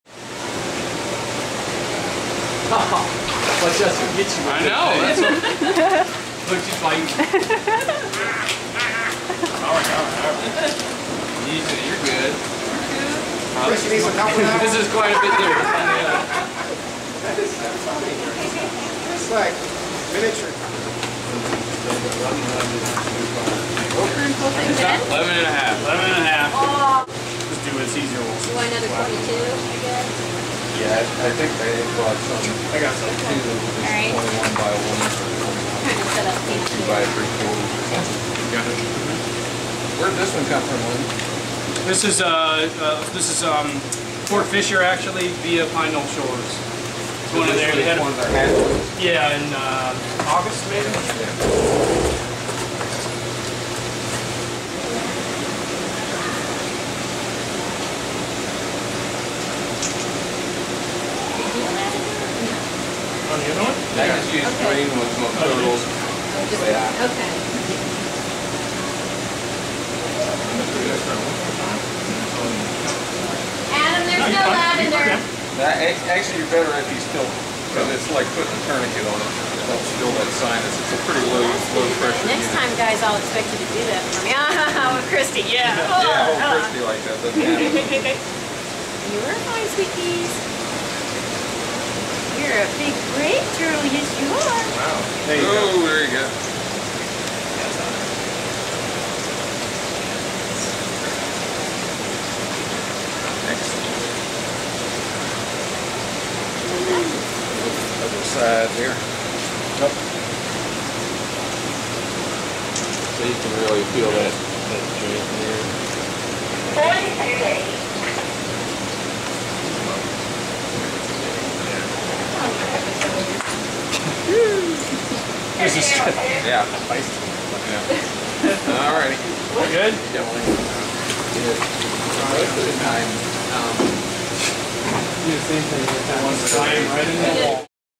Juvenile Sea Turtle Physical Exam at the Virginia Living Museum
Physical of the juvenile loggerhead sea turtle at the Virginia Living Museum in Newport News, VA